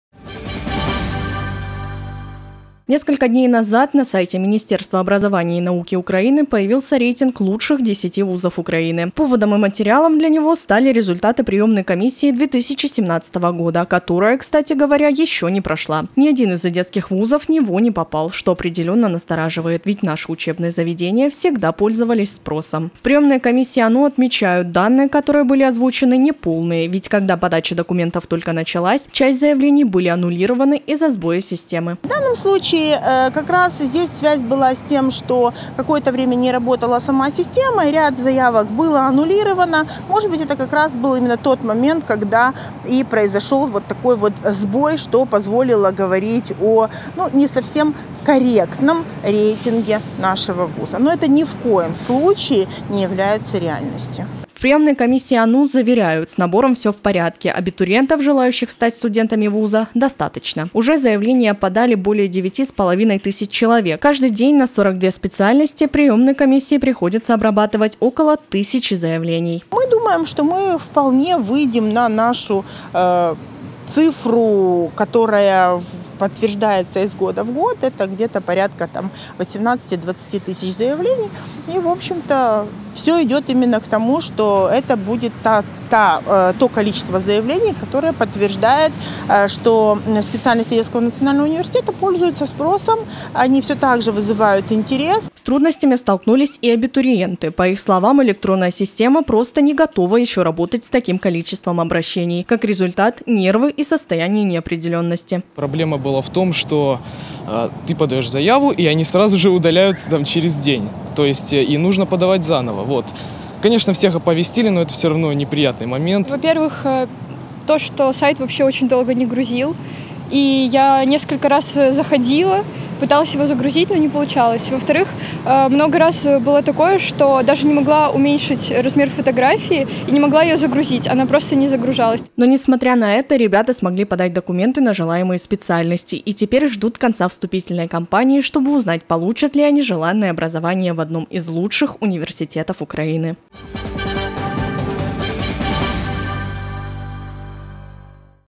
Те еще новости (21 07 17) в 10-ке или нет?
Не смотря на то, что вступительная кампания ещё не закончилась в министерстве образования и науки решили сделать рейтинг самых востребованных университетов Украины. Все бы хорошо, но вот только Одесские ВУЗы не попали даже в 10-ку. В Одесском национальном университете имени Мечникова возмущены – данные не объективны.